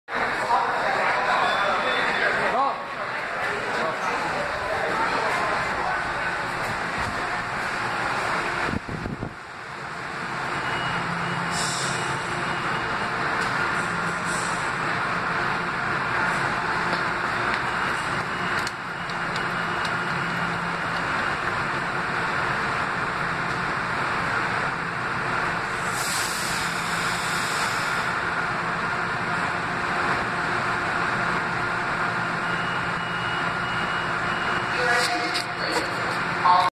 Metro north: New Canaan M8 train #1758 leaving Grand Central!
Here is a video of metro north train 1758 the 5:29PM Super Express out of Grand Central to New Canaan! With SINGLE M8 car #9562! Leaving from Track 102A!
New Haven line
Video taken January 8th 2016 at 5:29PM!